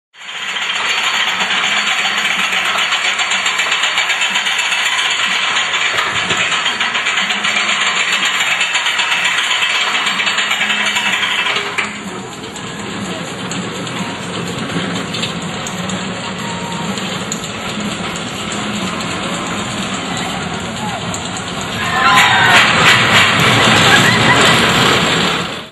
Junior Coaster
This is a simplified version of a Roller Coaster designed specially for the kids.The curves, incline and decline are being designed to add more excitement and fun for the users. The sounds during the operation and while breaking are similer to that of a train.